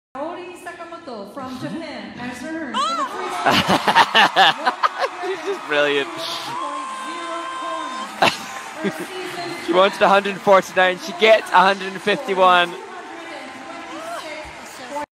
Kaori Sakamoto surprises herself at Skate Canada😁😂
Kaori Sakamoto lets out a cheer as she reacts to her winning free skate score at Skate Canada.